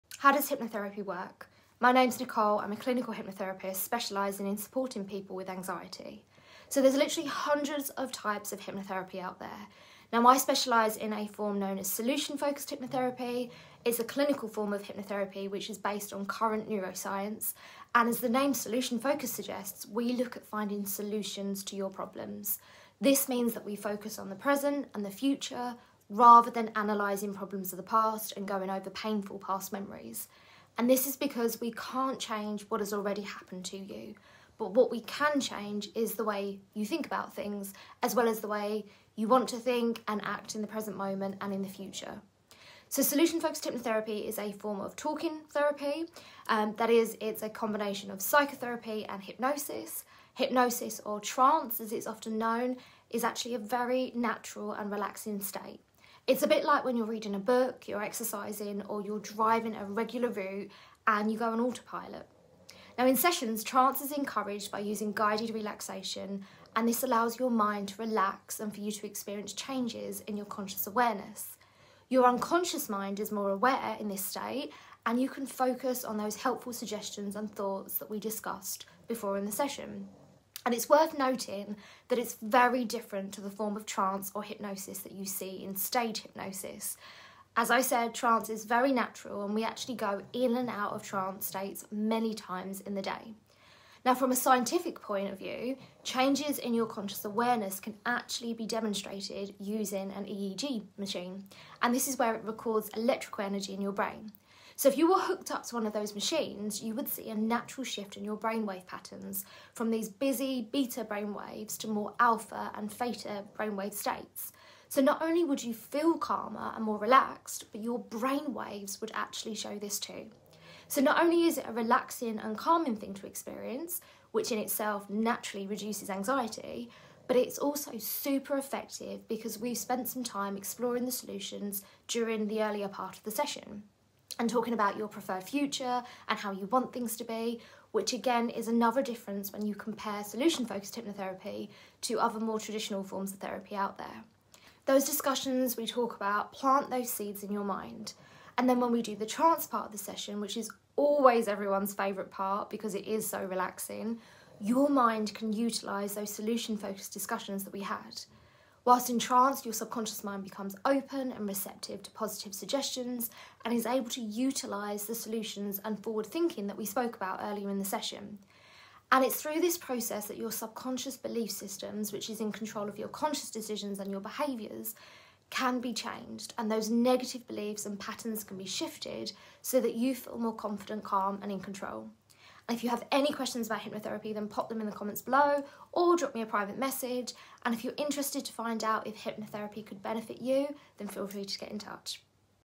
How Solution Focused Hypnotherapy works
In this video I explain what Solution Focused hypnotherapy is and how talk therapy is combined with hypnosis to shift those negative beliefs and change your subconscious mind (which is part of our brain that controls 95% of how we think, act and behave).

If you have any further questions drop them in the comments below or find out more over at: